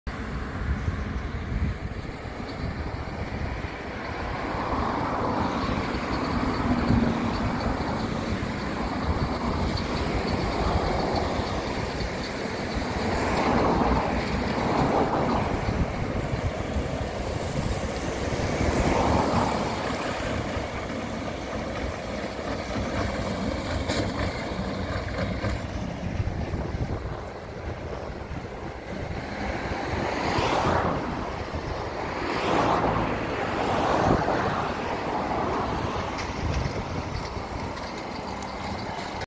Farming Tractor making big and beautiful Hay Bales
Hay is usually gathered up into bales, and stored for the animals to eat during the winter when the grass is not growing. In many parts of the world, farmers depend on hay to feed their cattle, sheep, or horses during the long winter period.
while i was walking I saw a farming tractor making hay bales in a big field. Wonderful to watch. I took the opportunity to film it.